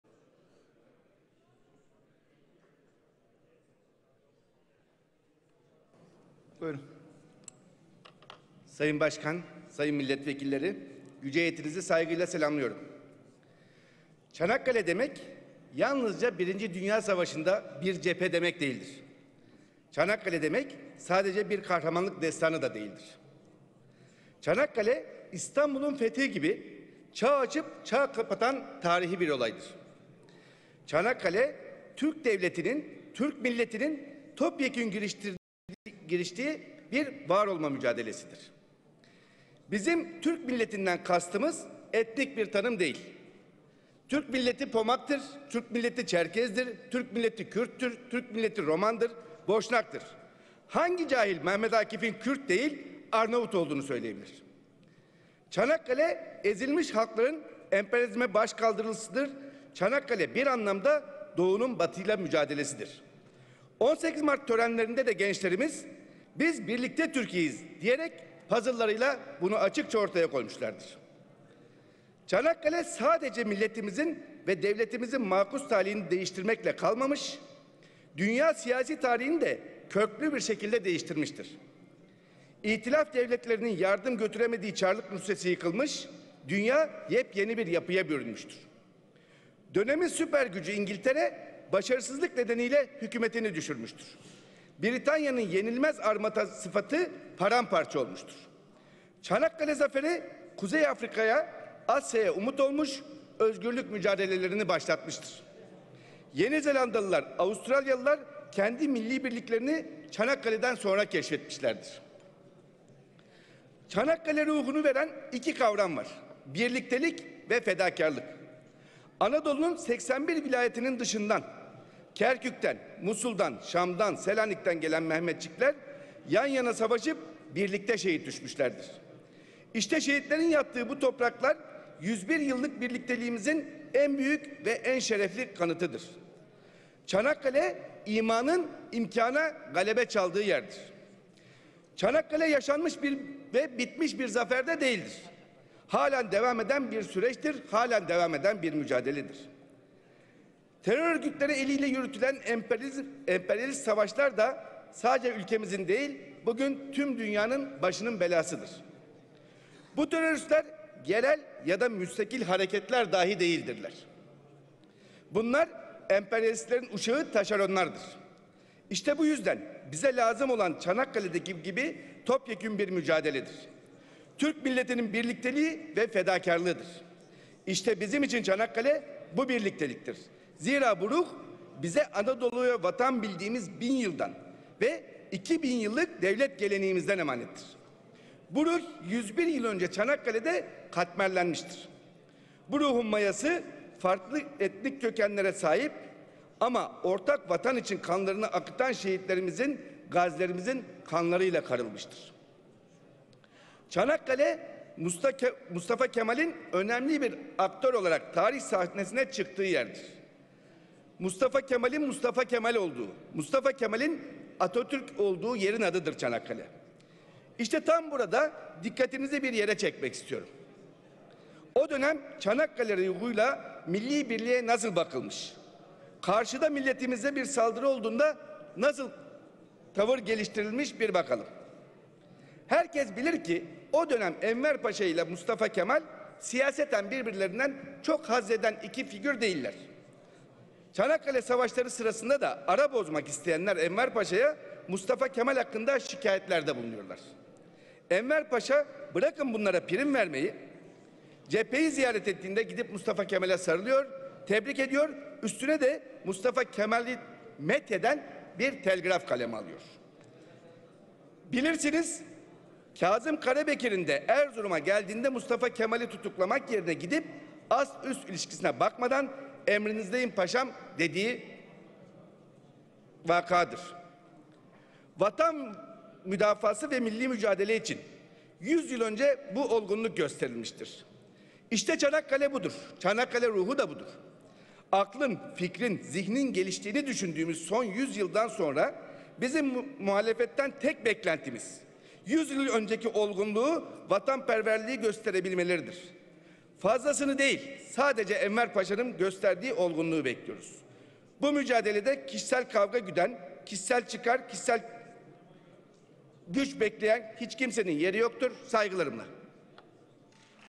Ayhan Gider - Çanakkale Ruhu - TBMM Genel kurulu
Çanakkale Milletvekilimiz Ayhan Gider'in 22 Mart  2016 Saat 15:15'te  TBMM Genel Kurulunda Çanakkale Ruhunu anlattığı bir Konuşma.